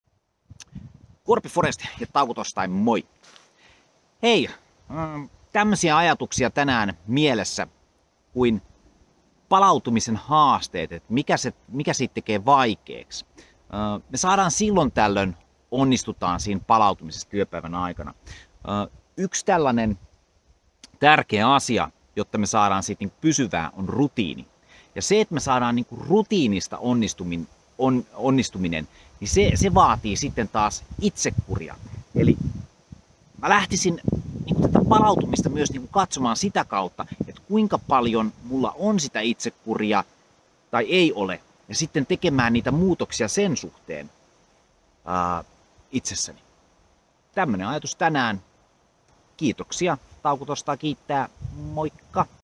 Taukotorstai itsekuria
taukotorstai ajatus

Palautuminen ei välttämättä ole se helpoin asia saada onnistumaan työpäivän aikana.

Haasteita tulee varmasti monesta suunnasta. Yksi asia, joka voi auttaa palautumisen juurruttamiseen on itsekuri!

Millainen sulla on itsekuri hallussa? Oisko trimmaamisen varaa?